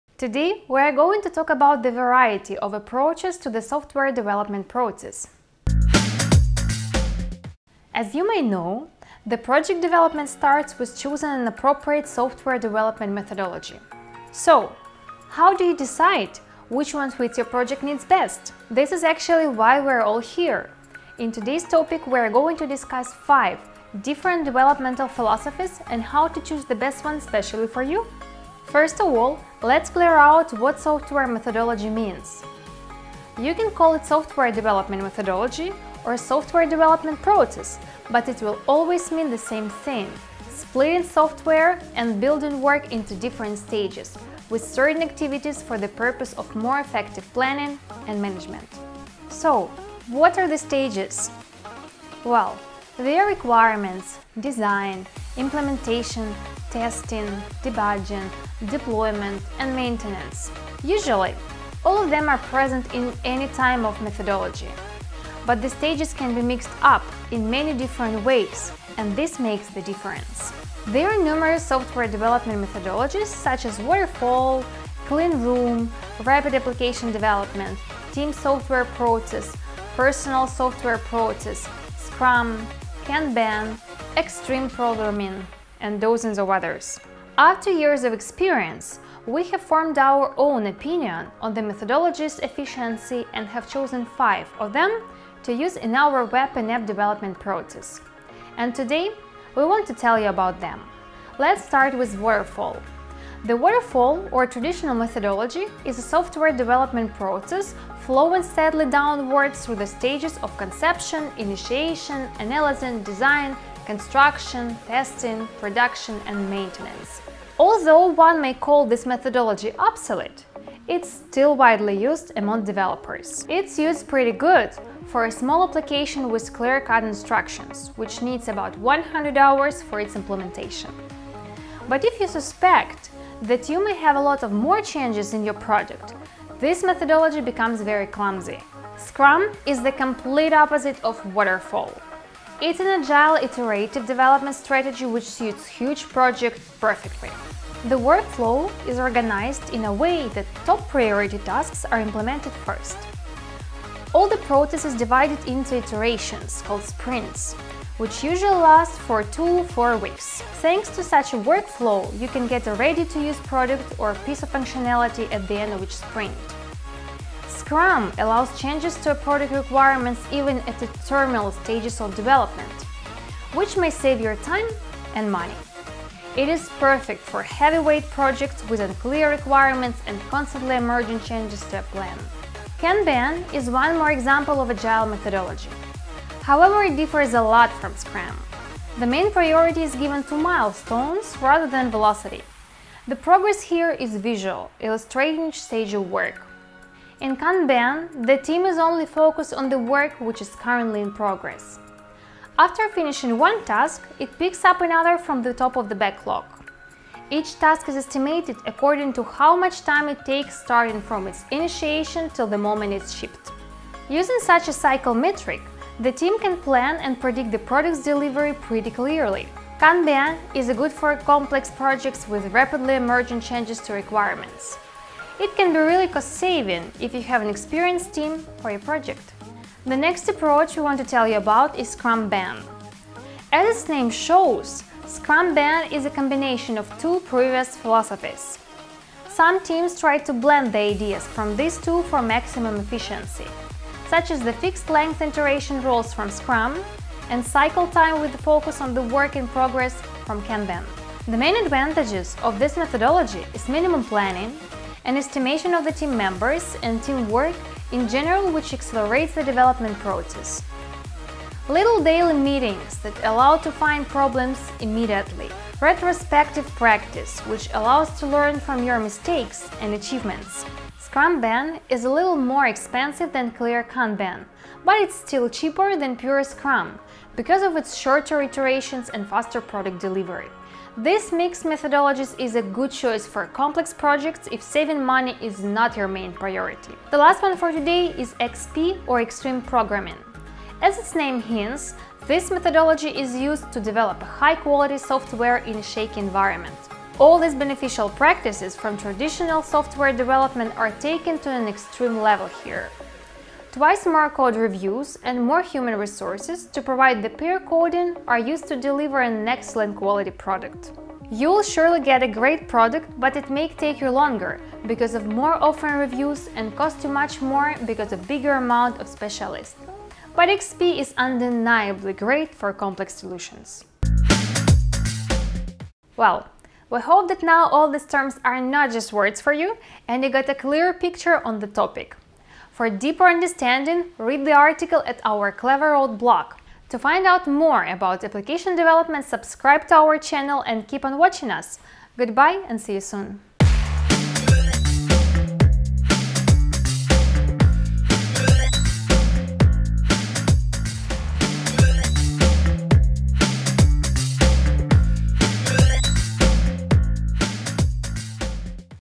What is the Best Software Development Methodology for Your Project?
Mobile apps development is a complex process which is based on methodologies. Let’s take a look at 5 different developmental methodologies to find out the most effective one to build your product. They are:
*Waterfall
*Scrum
*Kanban
*Scrumban
*Extreme programming